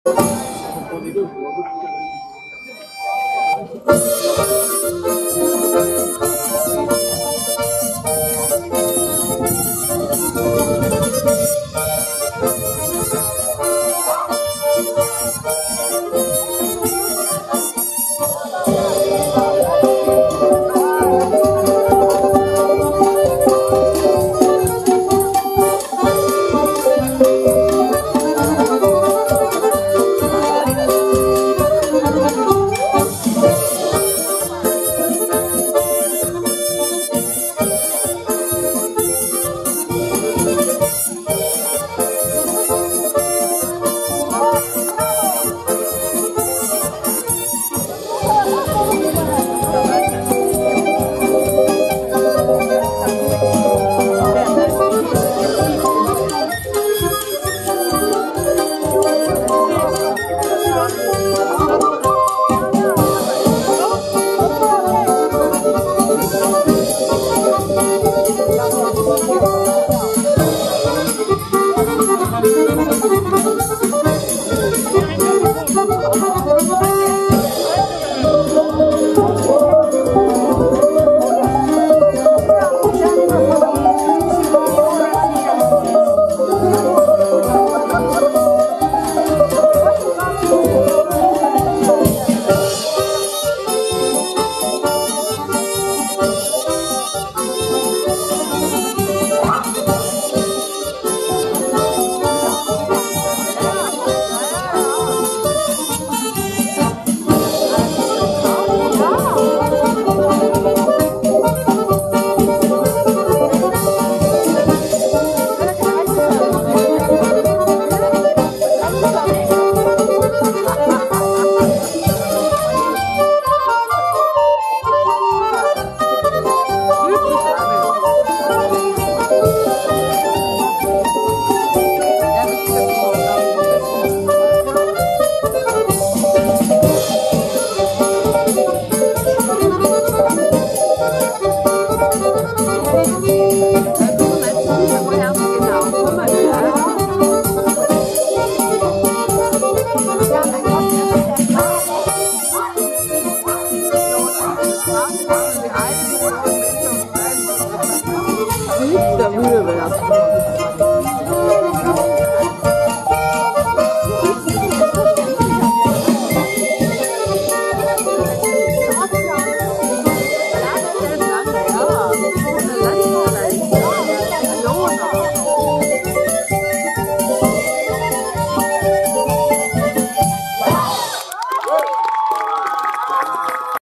Вологодская кадриль
В рамках Дней Старого города состоялось выступление коллективов Союза национально-культурных обществ " Лира"